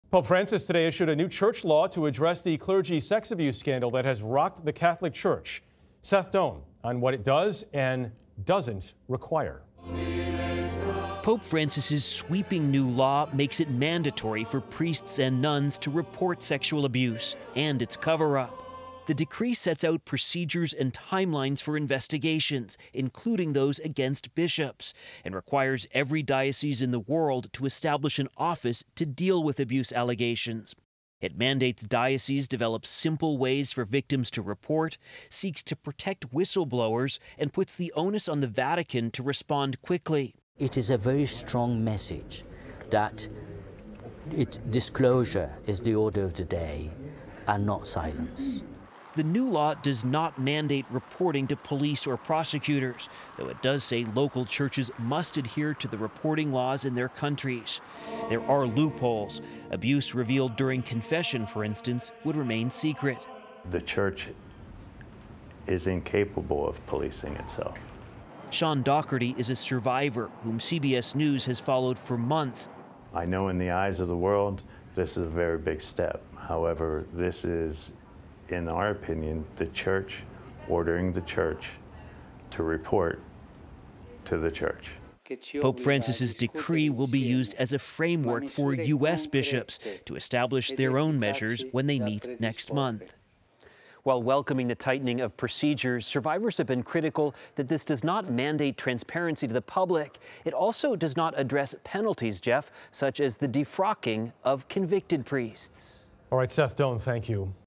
Pope Francis issues new Vatican law on reporting of sexual abuse
Pope Francis issued a new church law to address the sexual abuse scandal that has rocked the Catholic Church. But it does not mandate reporting to police. Seth Doane explains.